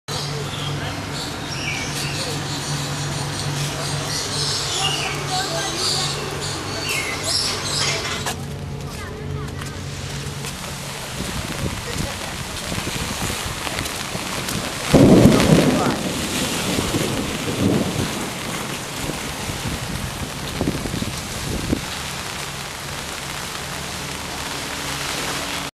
25 seconds at the Southern most point of continental Asia
Friends at home are familiar with the Southern most point of the continental USA, at Key West, Florida.  Here's the Southern most point of continental Asia, as seen in rain and thunder.